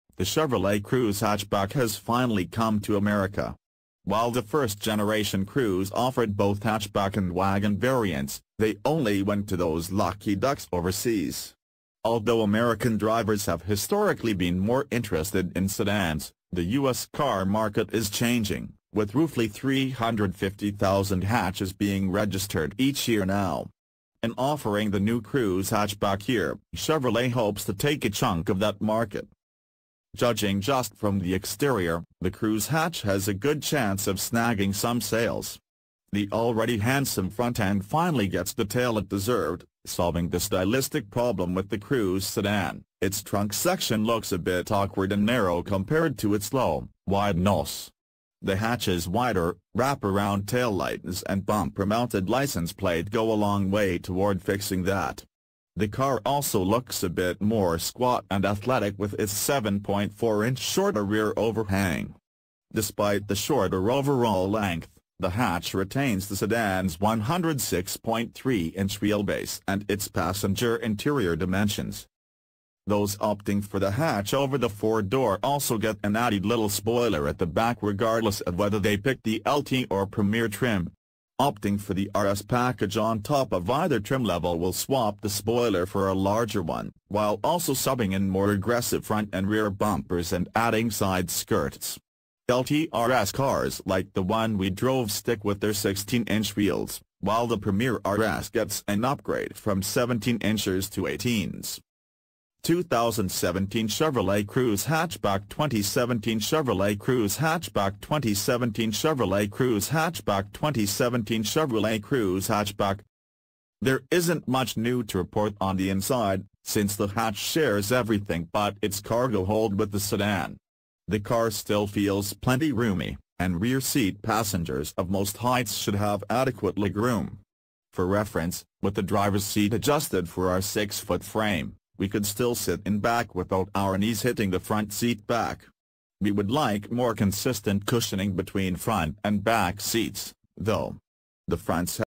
2017 Chevrolet Cruze Hatchback First Drive
The dashboard and other interior components are all put together nicely, with tight gaps and good fit. Every hatch comes with upholstered panels on the doors in either cloth or leather, which is a nice departure from the stitched or faux-stitched plastic panels that are so popular nowadays. All Cruzes also come with a touch–screen infotainment unit with either a 7.0-inch or 8.0-inch display.

So we've established that the hatch is the best-looking version of the Cruze, but how does it drive? If you ask the Chevy folks (and even if you don't), they'll use the phrase "fun to drive" to describe the hatch. In reality, this Cruze variant drives just as well as the sedan, but we wouldn't characterize either of them that way, at least not in the sporty sense. While most compact hatches get driver-focused tuning – often similar to a European suspension spec, but brought here – the Cruze basically mirrors its sedan counterpart.